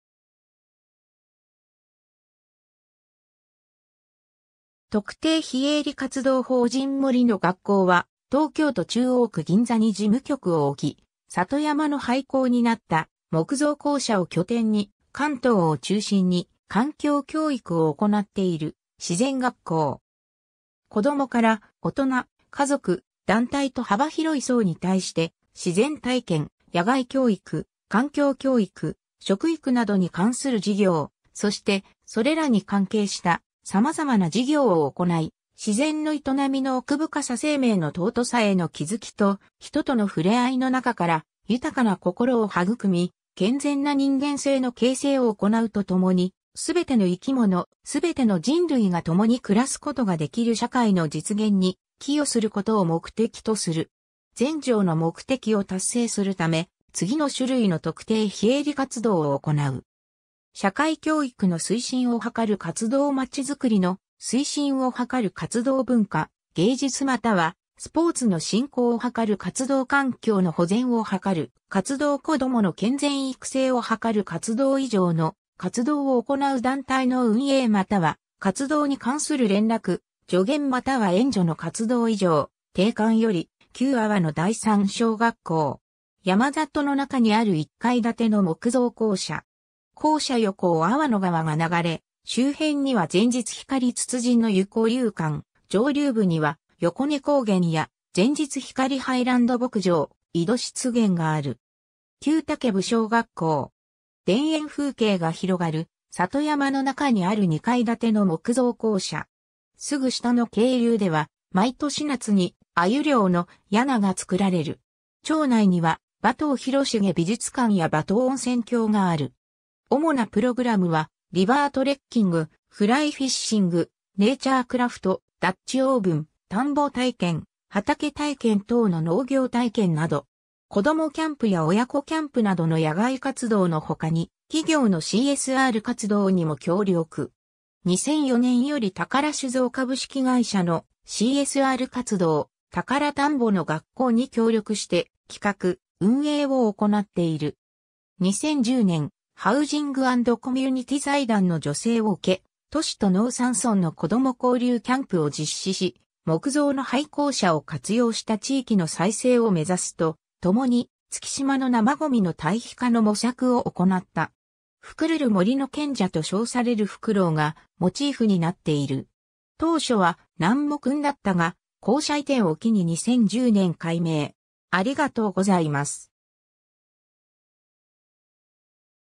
森の学校
特定非営利活動法人森の学校（もりのがっこう）は、東京都中央区銀座に事務局を置き、里山の廃校になった木造校舎を拠点に、関東を中心に環境教育を行っている自然学校。
 子どもから大人、家族、団体と幅広い層に対して、自然体験、野外教育、環境教育、食育などに関する事業、そして、それらに関係した様々な事業を行い、自然の営みの奥深さ生命の尊さへの気づきと、人との触れ合いの中から、豊かな心を育み、健全な人間性の形成を行うと共に、すべての生き物、すべての人類がともに暮らすことが出来る社会の実現に寄与することを目的とする。
前条の目的を達成するため、次の種類の特定非営利活動を行う。
 社会教育の推進を図る活動 まちづくりの推進を図る活動 文化、芸術又はスポーツの振興を図る活動 環境の保全を図る活動 子どもの健全育成を図る活動 以上の活動を行う団体の運営又は活動に関する連絡、助言又は援助の活動 以上、定款より 旧粟野第三小学校（栃木県鹿沼市入粟野667-1）。
山里の中にある一階建ての木造校舎。
校舎横を粟野川（下流で思川と合流）が流れ、周辺には前日光つつじの湯交流館、上流部には横根高原や前日光ハイランド牧場、井戸湿原がある。
 旧健武小学校（栃木県那須郡那珂川町健武2077-3）。
田園風景が広がる里山の中にある二階建ての木造校舎。
すぐ下の渓流では毎年夏に鮎漁の“やな”がつくられる。
町内には馬頭広重美術館や馬頭温泉郷がある。
 主なプログラムは、リバートレッキング、フライフィッシング、ネイチャークラフト、ダッチオーブン、田んぼ体験・畑体験等の農業体験など。
 子どもキャンプや親子キャンプなどの野外活動（アウトドア）のほかに、企業のCSR活動にも協力。
2004年より宝酒造株式会社のCSR活動「TaKaRa田んぼの学校」に協力して企画・運営を行っている。
 2010年、ハウジングアンドコミュニティ財団の助成を受け、都市と農山村の子ども交流キャンプを実施し、木造の廃校舎を活用した地域の再生をめざすとともに、月島の生ゴミの堆肥化の模索を行った。
 「ふくるる」 森の賢者と称されるふくろうがモチーフになっている。
当初は「なんもくん」だったが、校舎移転を機に2010年改名。